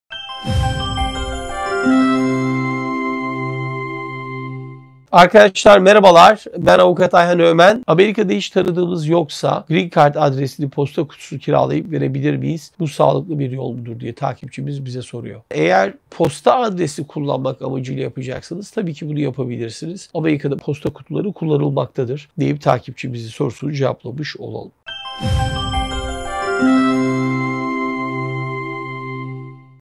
GREEN CARD İÇİN AMERİKA’DA POSTA KUTUSU KİRAYALABİLİR MİYİM ?
Soru: Amerika’da hiç tanıdığımız yoksa Green Card adresini posta kutusu kiralayıp verebilir miyiz ? Bu sağlıklı bir yol mudur ?